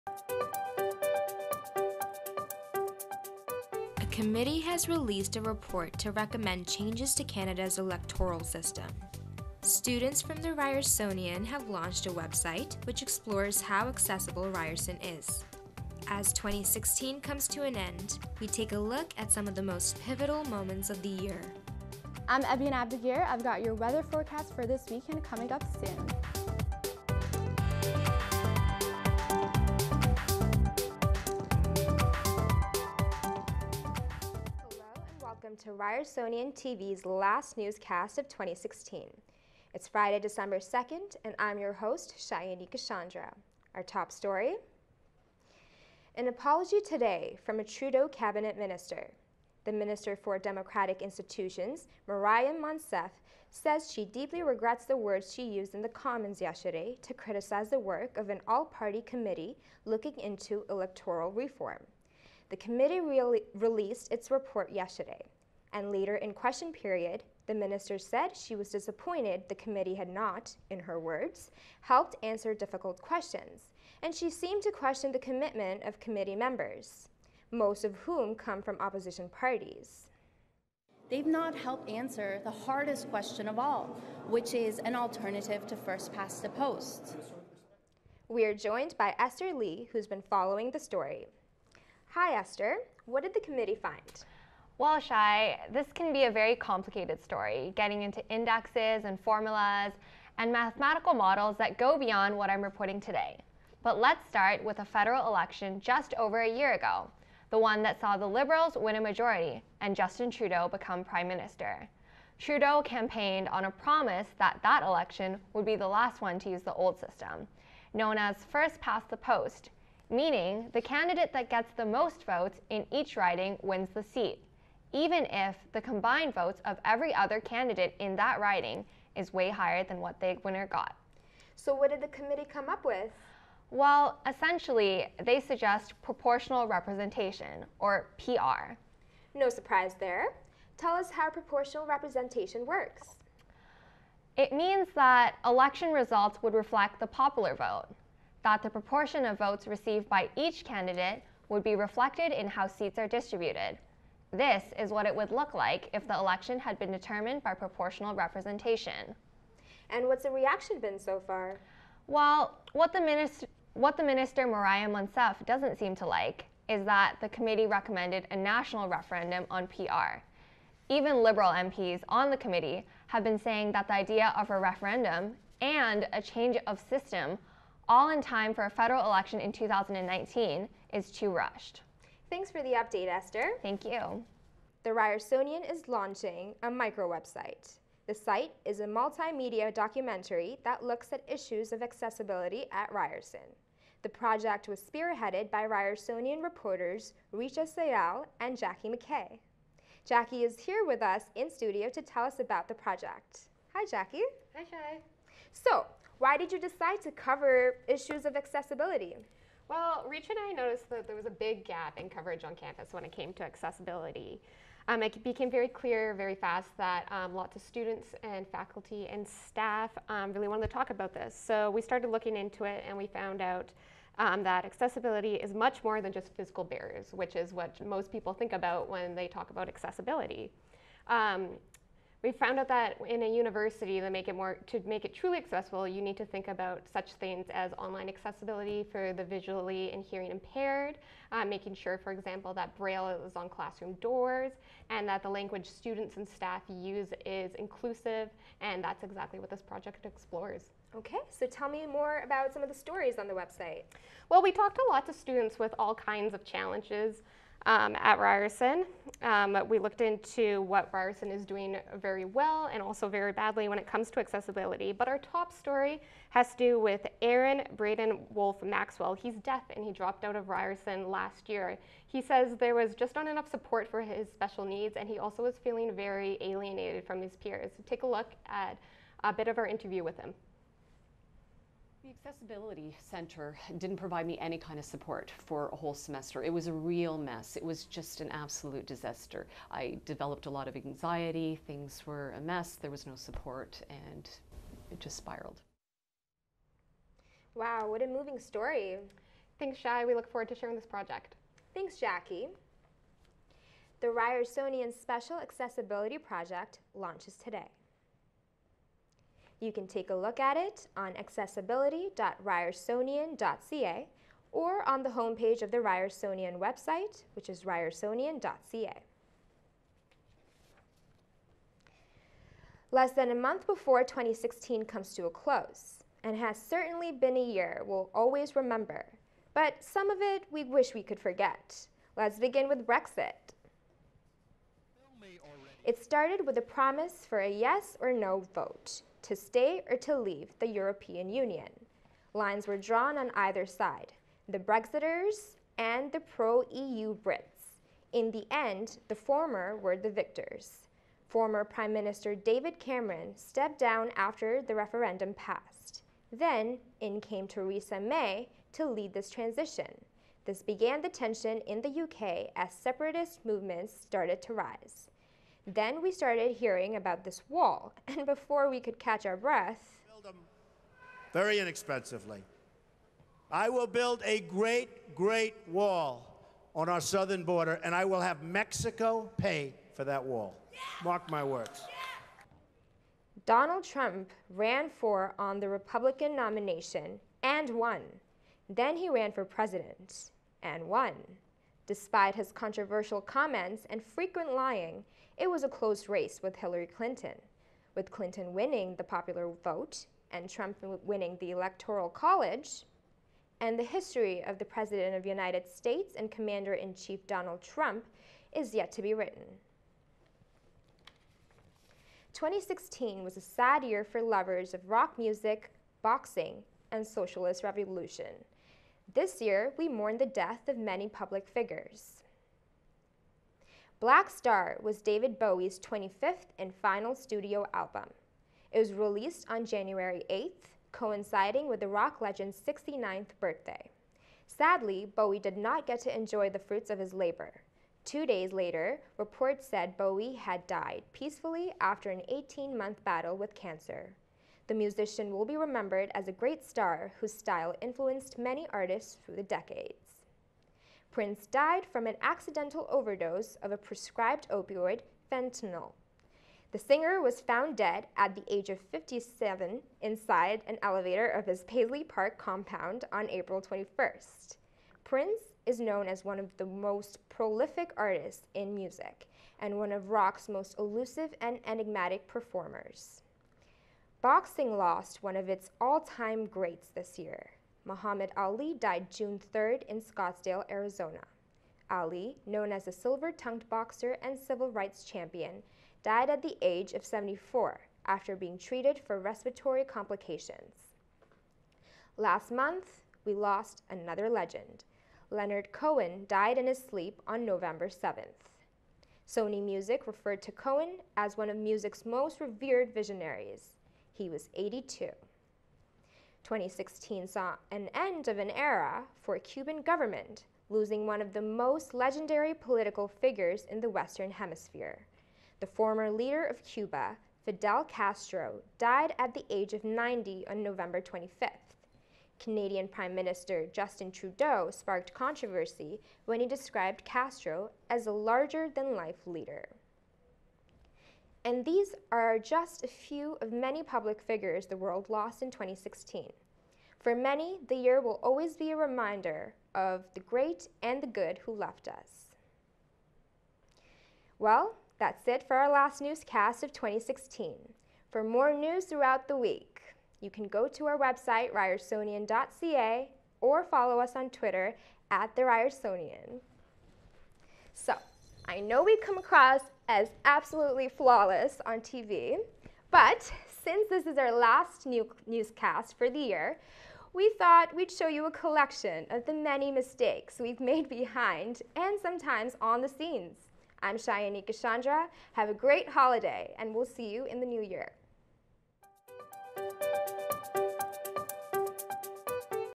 RyersonianTV: Dec. 2/2016 - ANCHOR DEMO
RyersonianTV is a news broadcast done by The Ryersonian's masthead. For more of Chayonika Chandra's work, please check out